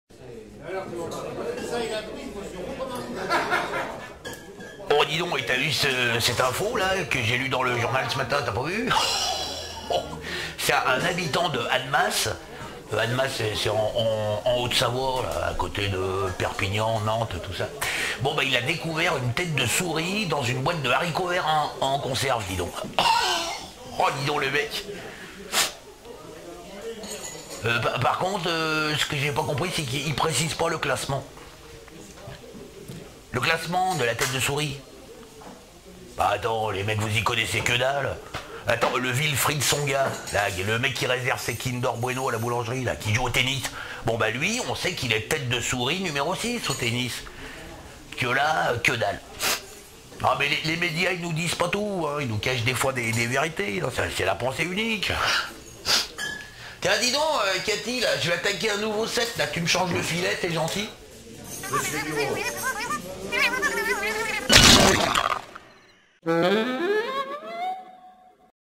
Brève de comptoir - Il découvre une tête de souris dans ses haricots verts en conserve
Un Savoyard a découvert une tête de souris dans ses haricots verts en conserve.
Une info insolite qui fait causer dans les bistros !!